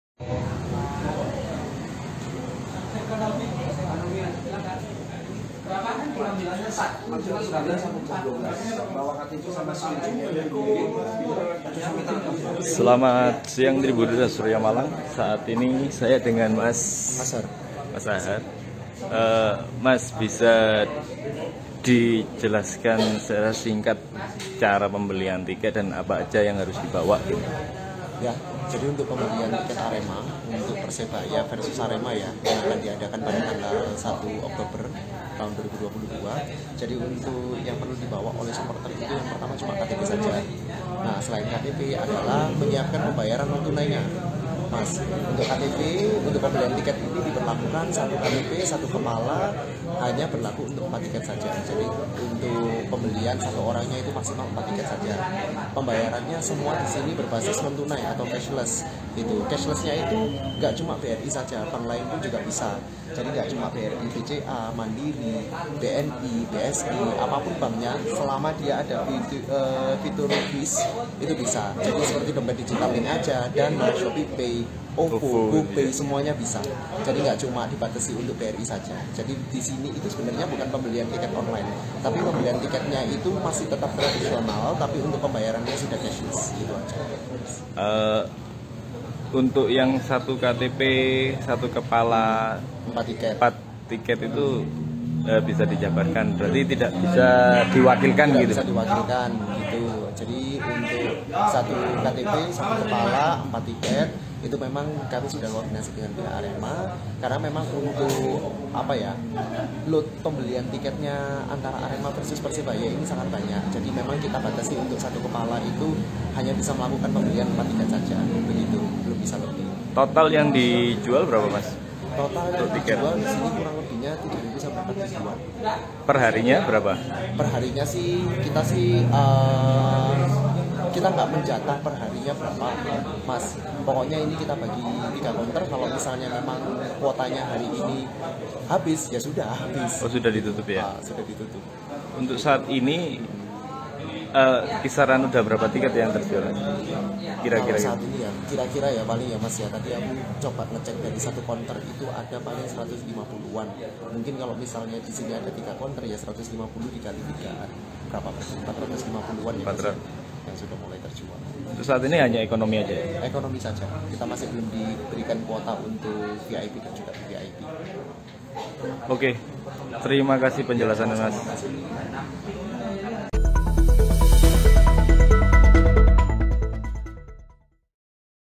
Ini Cara Pembelian Tiket Arema Dengan Metode Scan QRIS
Reporter : Rizal Van Nani - Editor Video : Akhmad Yusuf
MALANG - SURYAMALANG.COM

Per KTP, 1 Kepala mendapatkan 4 tiket